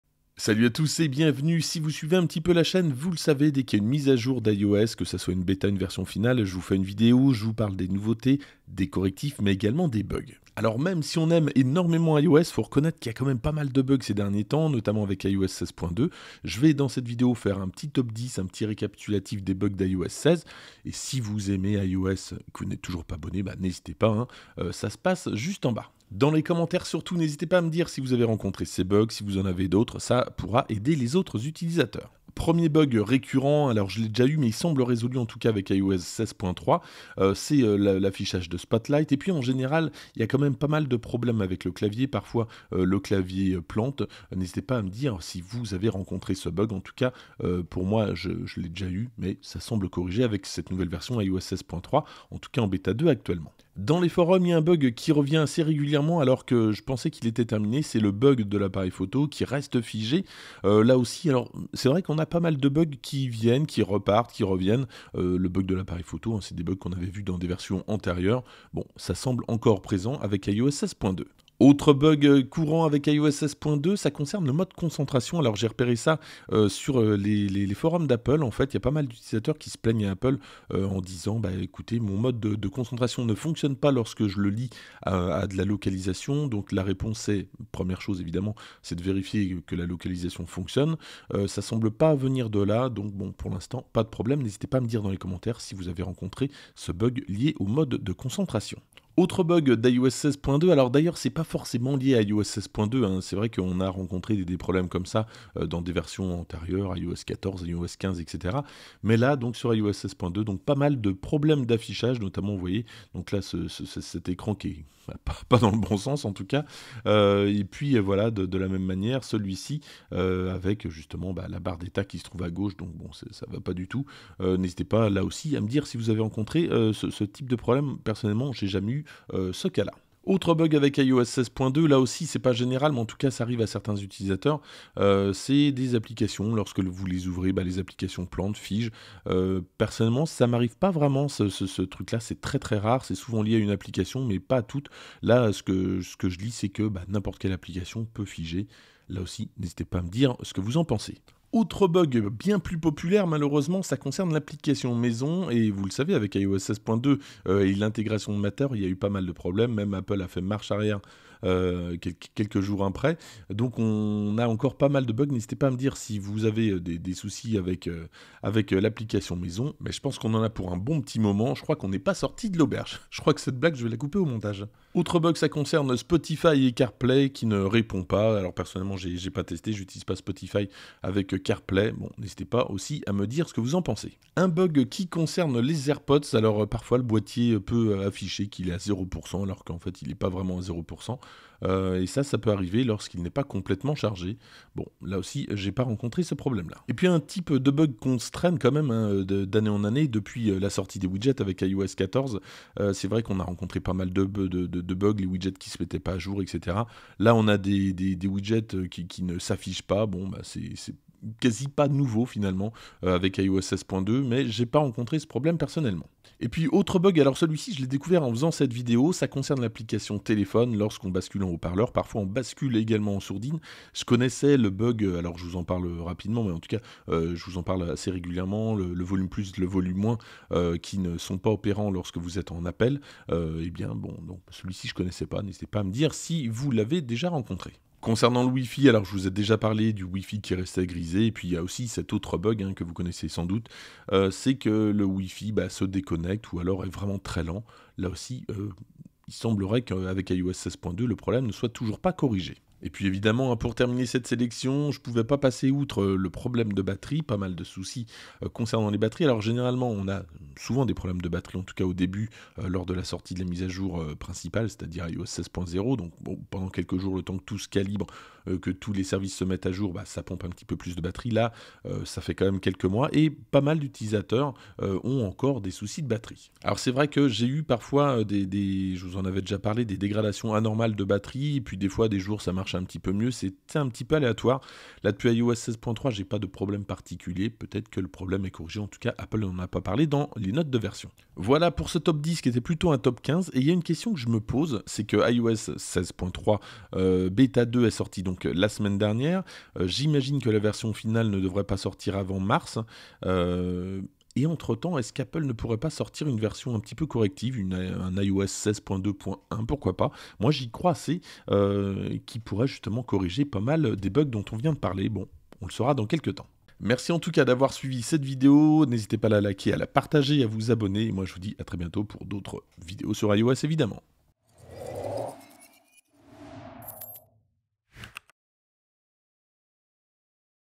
Bugs iOS 16 et iOS 16.2 : Avez vous l'un de ces 15 bugs sur votre iPhone?
Depuis qu'Apple a sorti iOS 16, pas mal d'utilisateurs ce plaignent de bugs et toujours avec iOS 16.2. Dans cette vidéo je fais un récapitulatif de certains bugs sur iPhone .
La boutique Apple sur Amazon